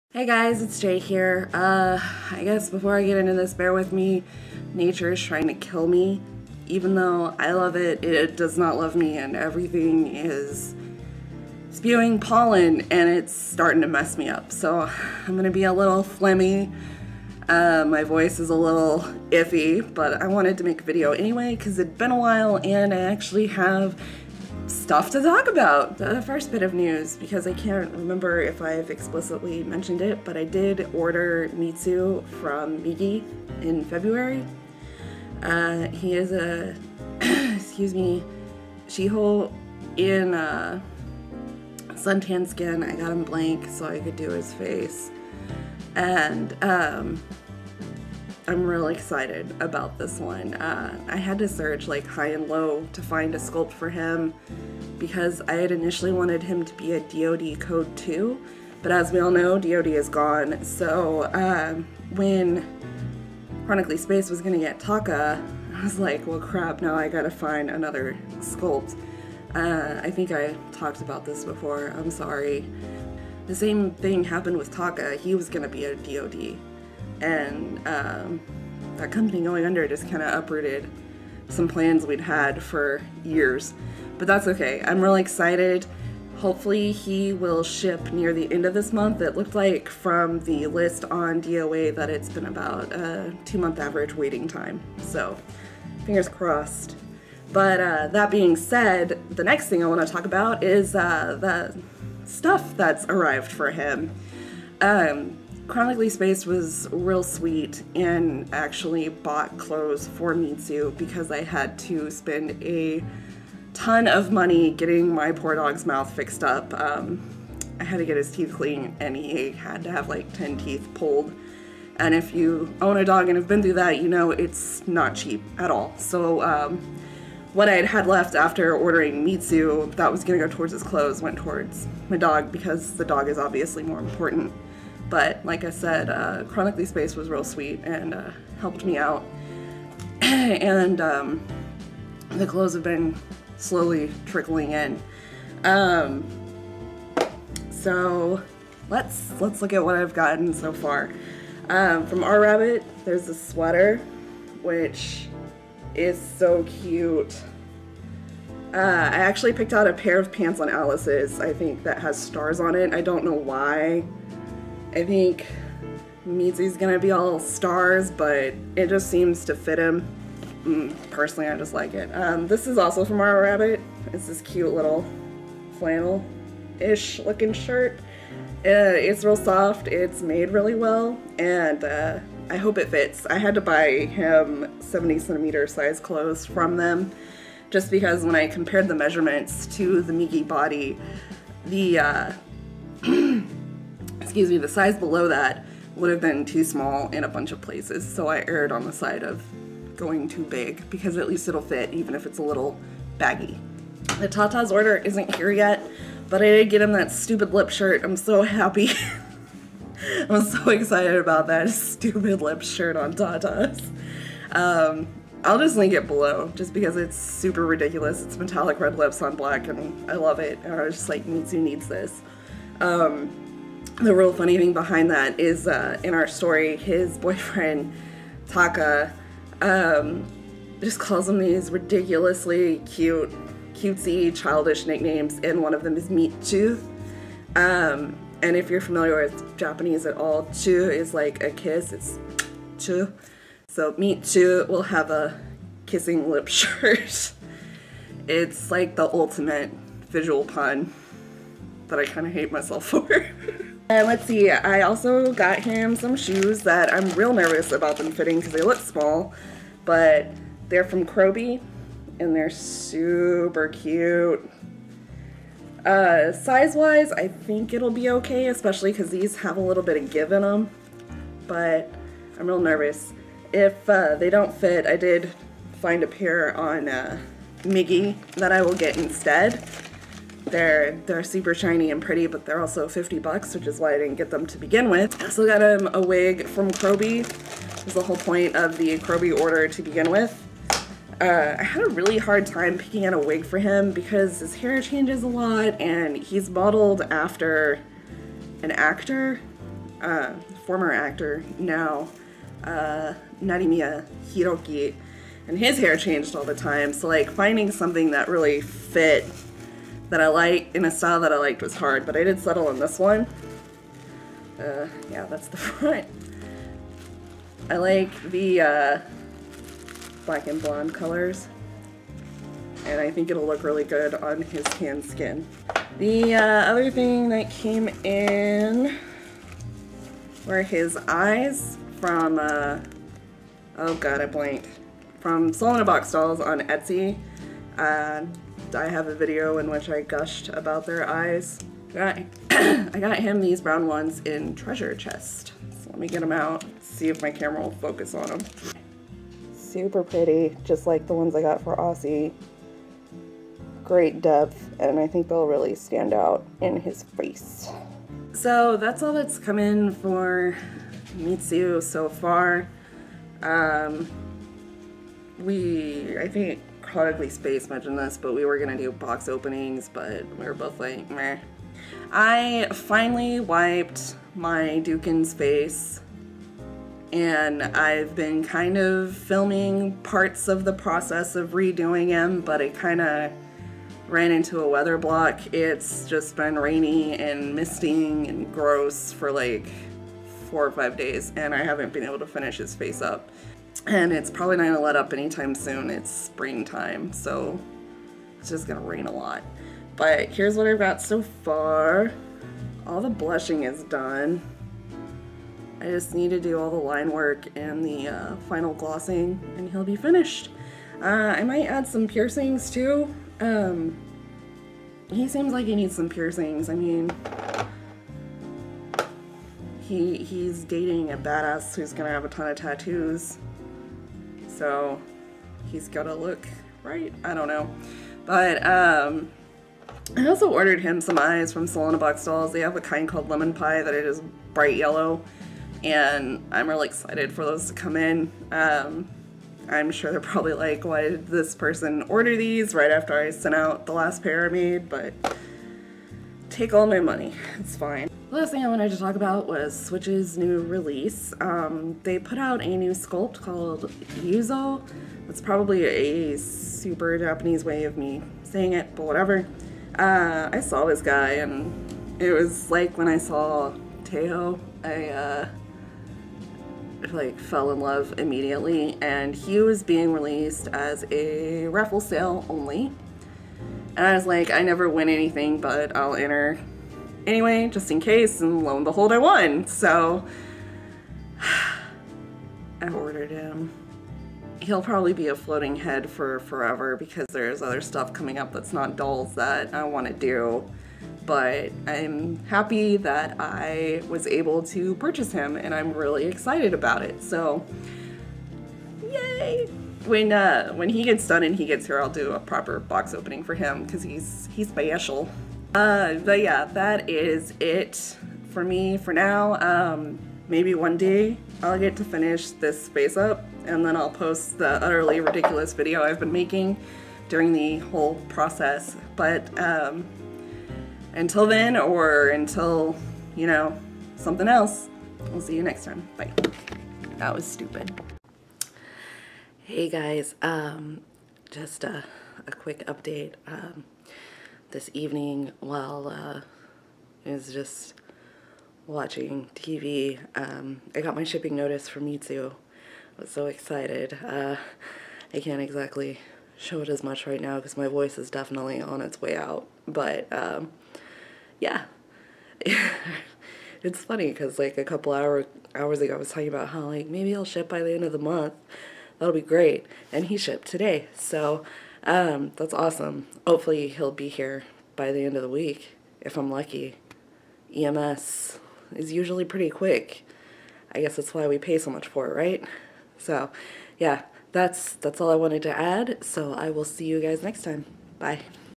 April Dolly Updates
A video that's all over the place and fueled by my determination to thwart the forces of nature that are stealing my voice. Sorry for the choppy editing, I tried to get most of the coughing out of there!

Today, I talk about clothes that have come in for the new boy, current projects, and an unexpected order.

(Sorry for my voice, it's pretty much done).